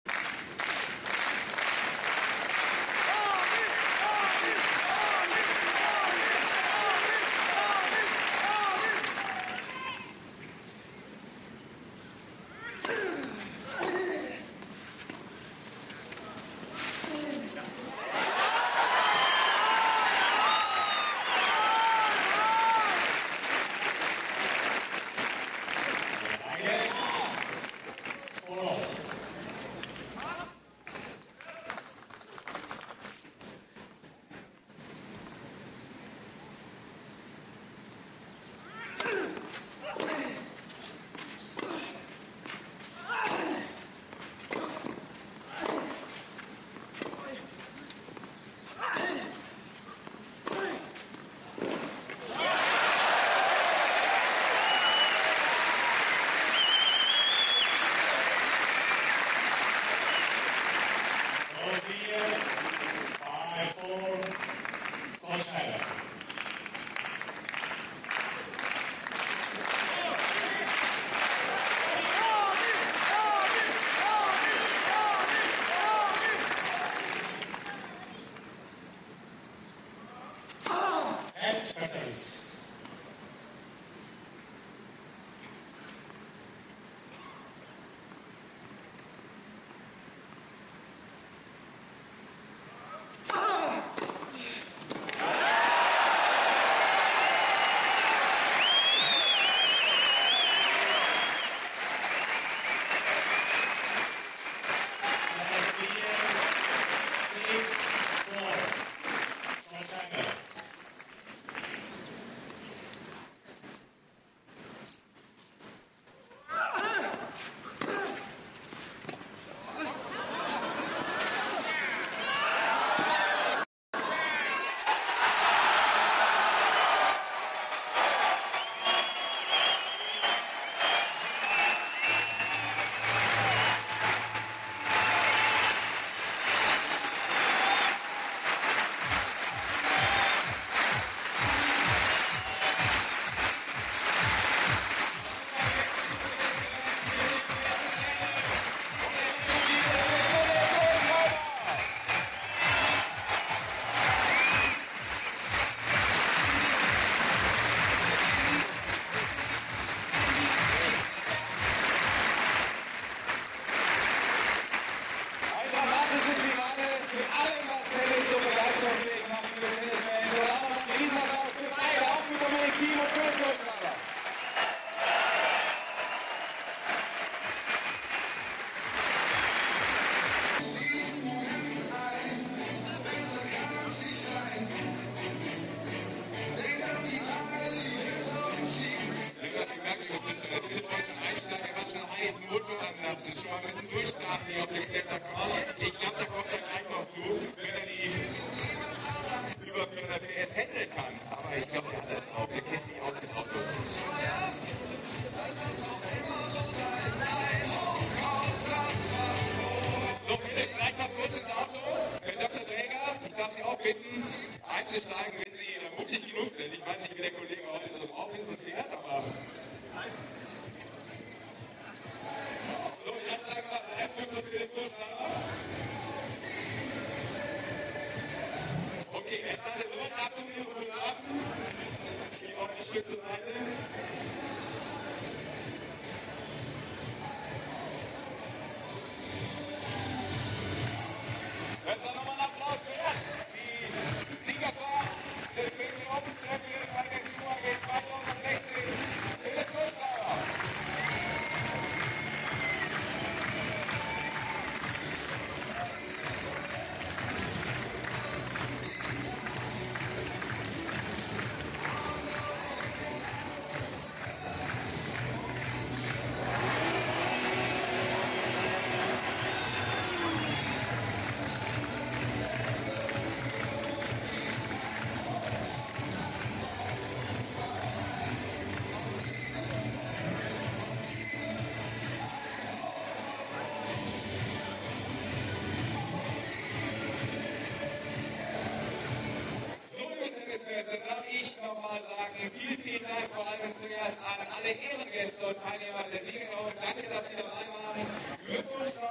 ATP Munich FINAL (01.05.2016 // by LTV)
250 SERIES BMW Open by FWU AG (Finals)
Philipp Kohlschreiber (Germany) 2-1 Dominic Thiem (Austria)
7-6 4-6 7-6